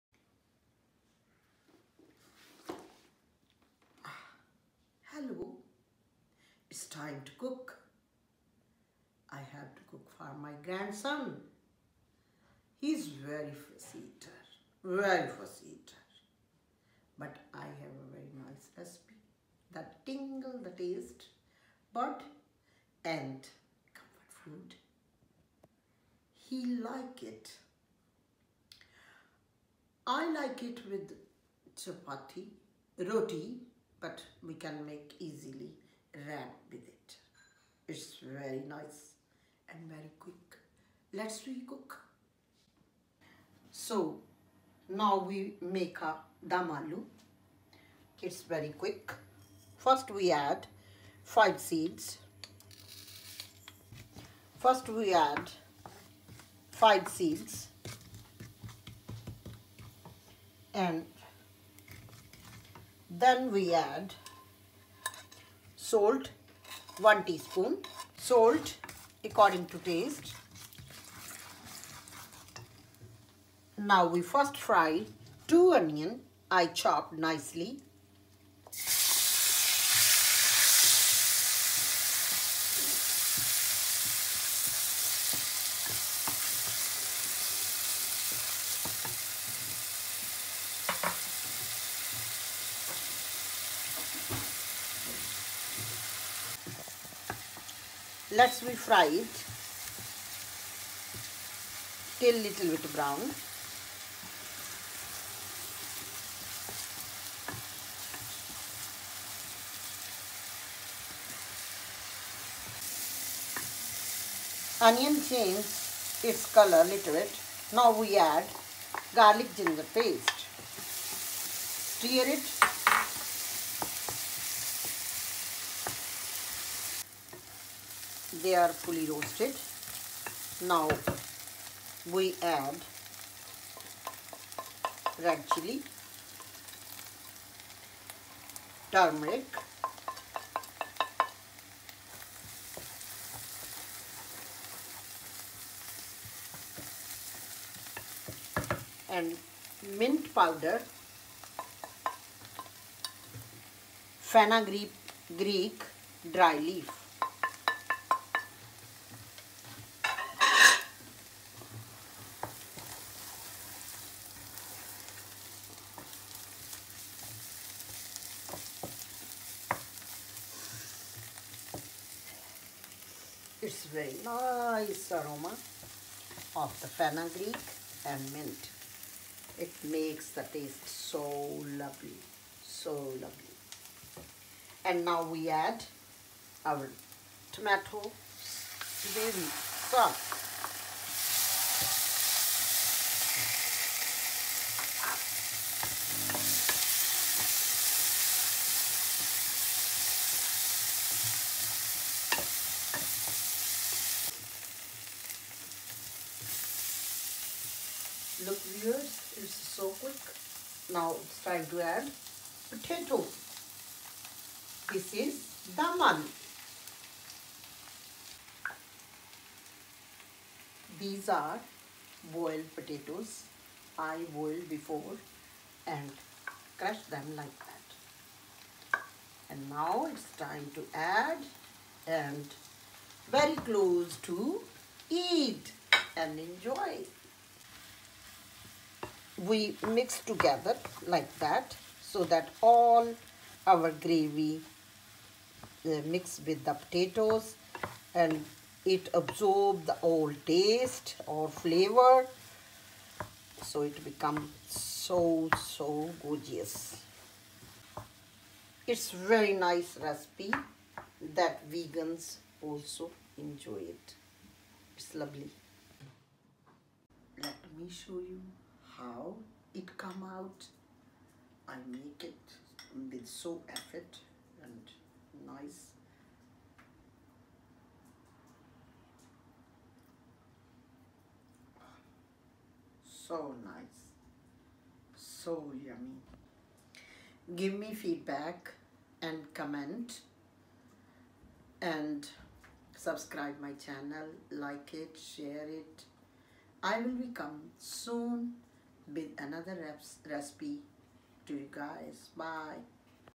Dum Aloo Recipe/ Punjabi Mash Potato
Dum Aloo or Indian Mash Potato easy recipe in 6 minutes. Vegan recipe ideal for children.